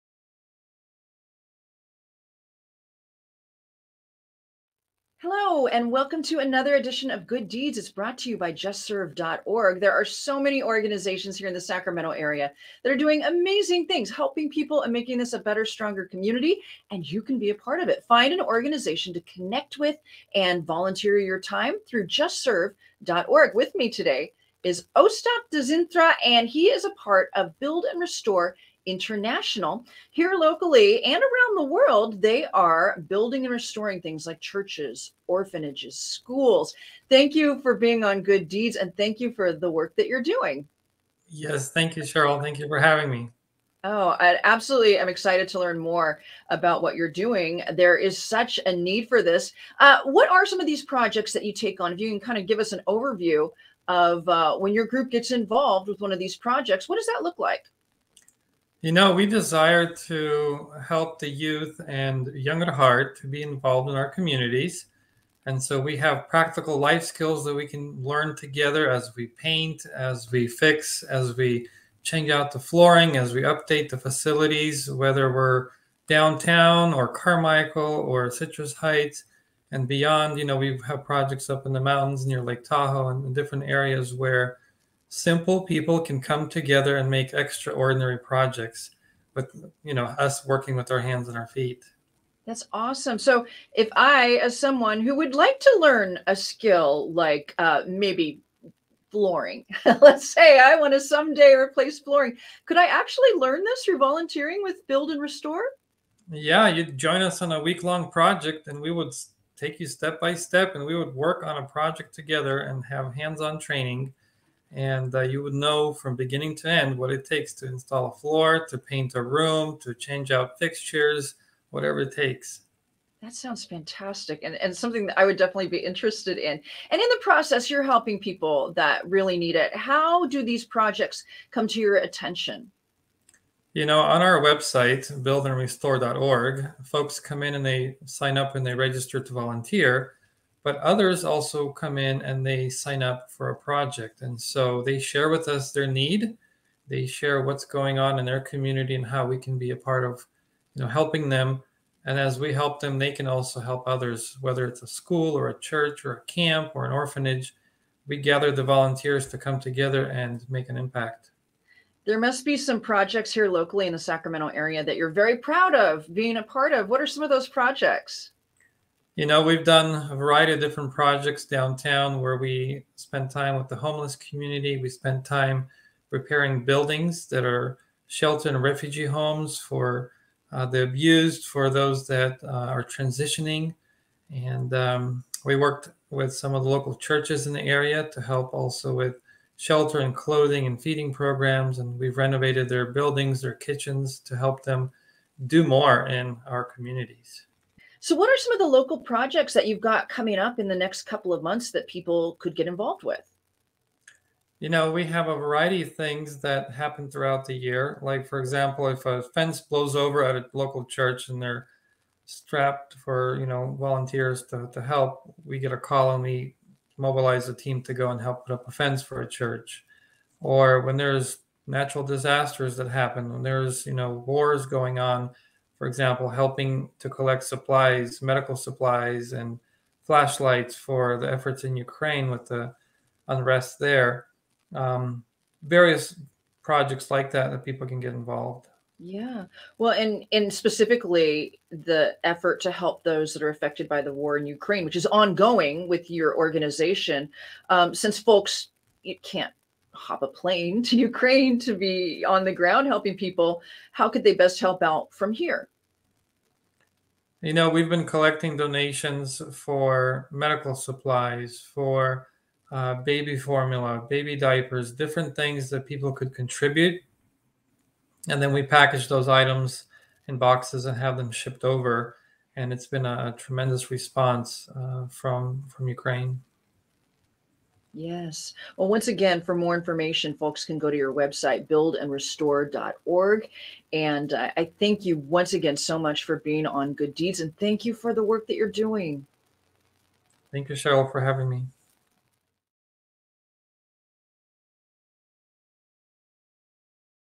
Sheryl's Good Deeds With Build & Restore
Each week we team up with JustServe.org to motivate service in the community.  This week, we learn more about Build & Restore International. They organize and implement construction restoration projects for churches, schools, and in some cases homes of those hit by natural disasters. They are looking for volunteers to help with some upcoming local projects, as well as support for their ongoing efforts to help those affected by the war in Ukraine.